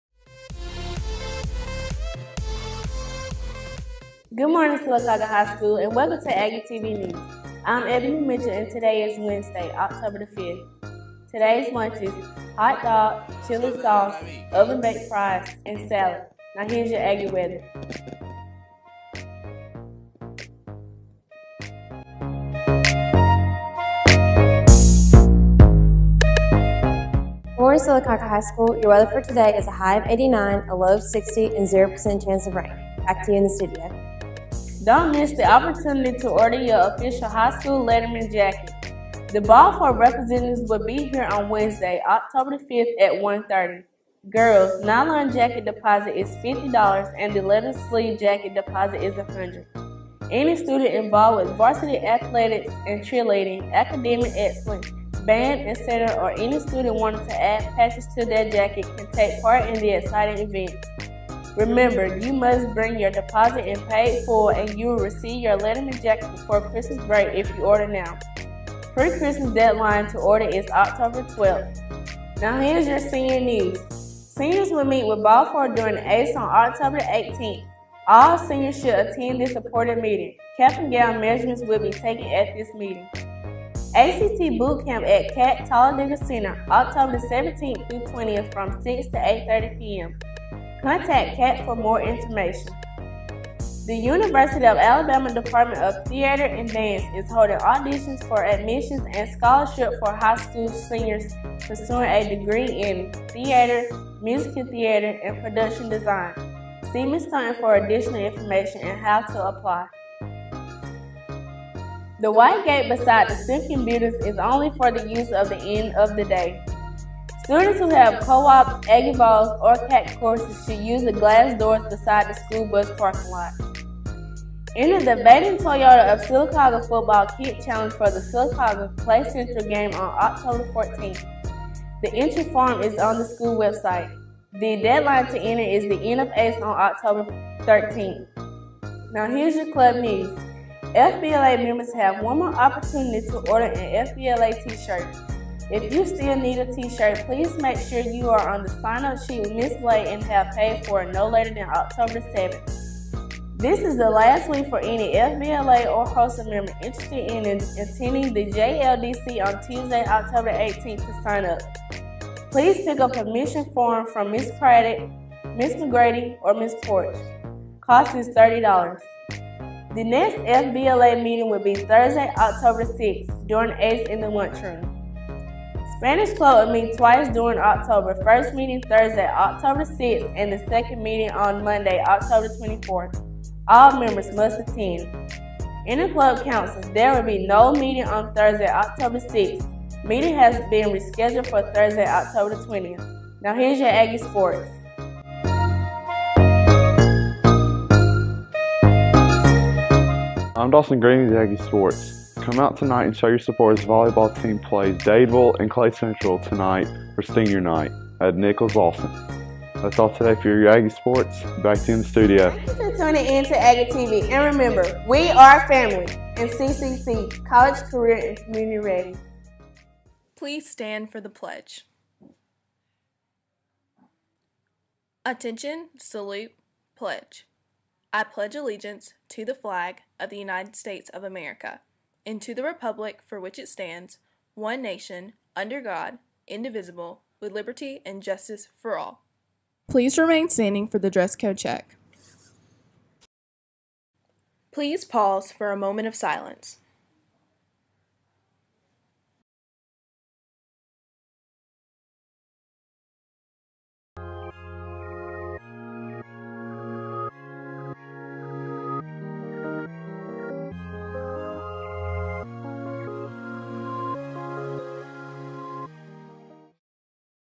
Aggie TV 10/5/2016
This is your daily dose of Aggie TV! Enjoy! Today's Cast: Rylee Jones, Ebony Mitchell, and Dawson Green.